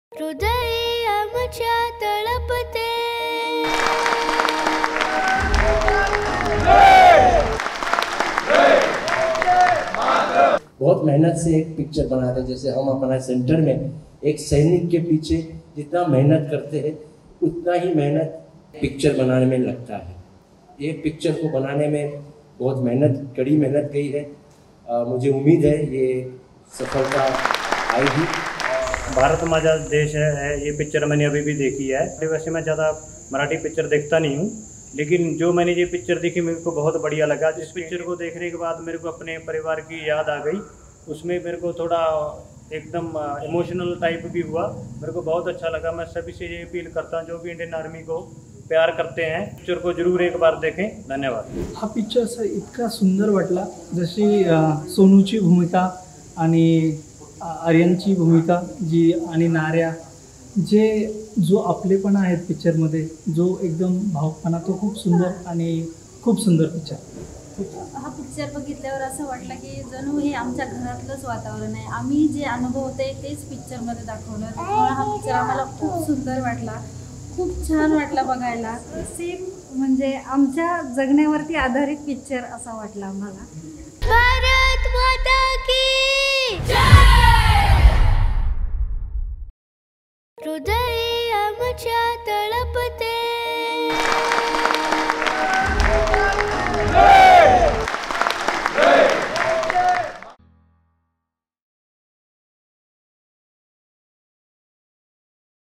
'Bharat Maza Desh Aahe' हा चित्रपट पाहिल्यानंतर कुटुंबियांची आठवण आली' | LetsUpp Marathi
बहुचर्चित चित्रपट 'भारत माझा देश आहे' अखेर चित्रपटगृहात प्रदर्शित झाला. या चित्रपटाचा प्रिमिअर शो कर्नाटकातील बेळगाव इथं सैन्य अधिकारी, तसेच जवानांसह त्यांच्या कुटुंबियांसाठी आयोजित करण्यात आला होता. हा चित्रपट पाहिल्यानंतर जवानांनी आणि त्यांच्या कुटुंबियांनी प्रतिक्रिया दिल्या आहेत. यावेळी हा चित्रपट पाहिल्यानंतर आमच्या कुटुंबियांची आठवण येत असल्याचं जवान म्हणाले.
स्रोत - लेट्सअप न्यूज नेटवर्क
प्रोड्युसर : अमोल भिंगारदिवे
व्हिडिओ एडिटर : प्रशांत कांबळे